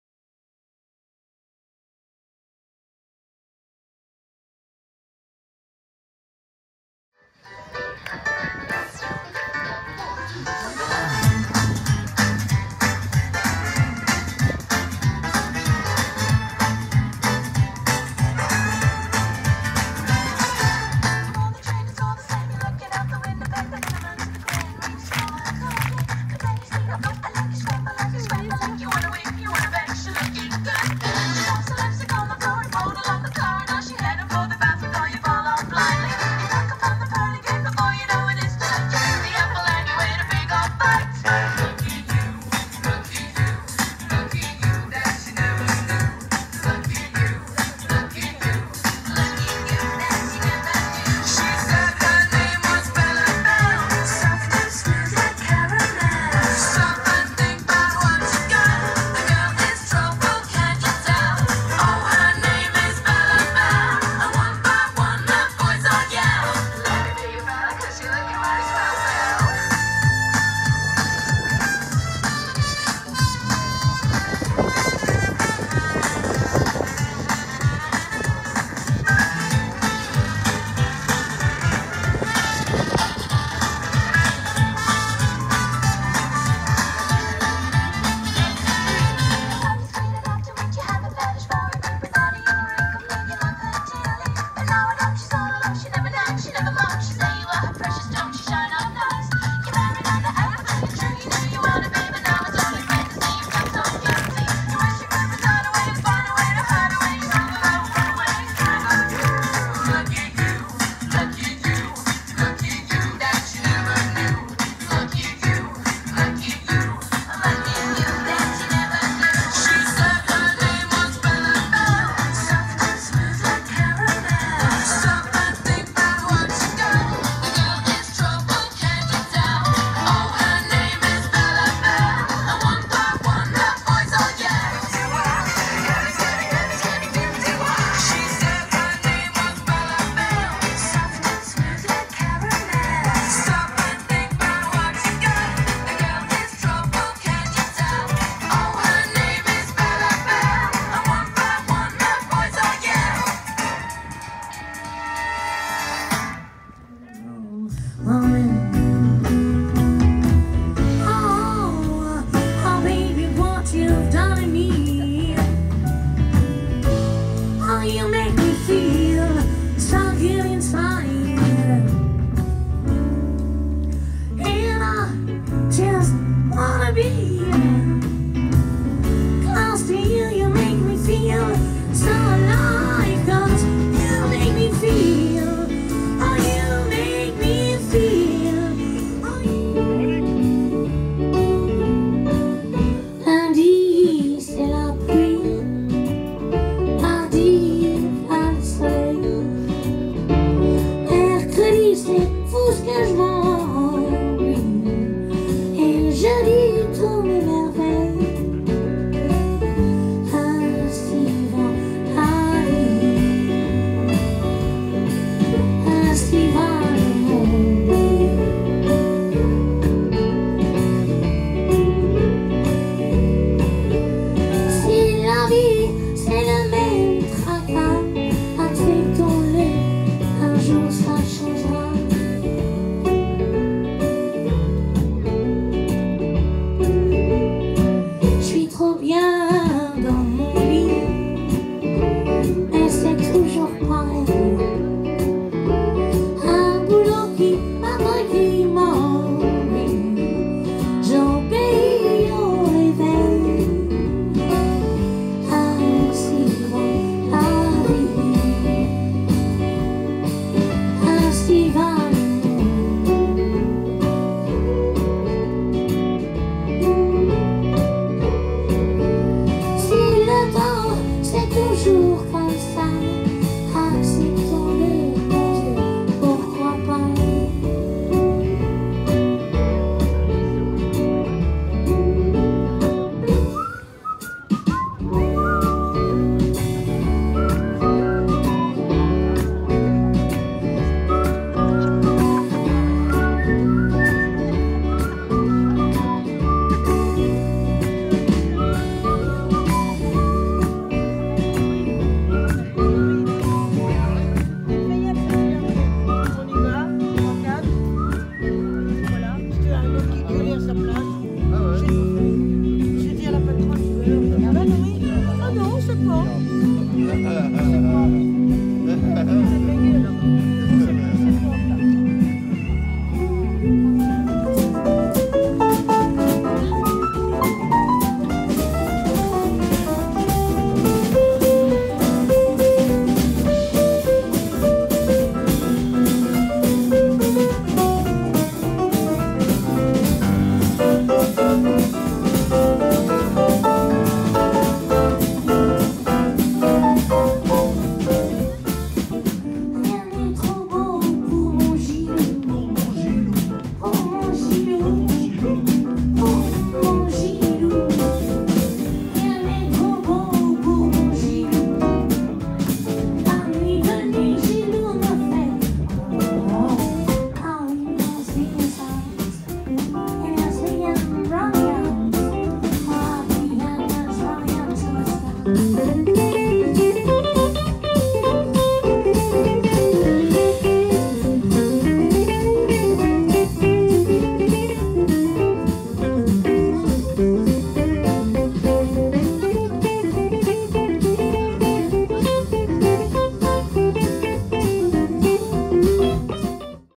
Royan 18 Août Audrey et les faces B & Henry Carillo
Animation avec Henry Carillo Routine Bella Belle suivi de Audrey et les faces B Concerts -Spectacles -Animations CFAR Royan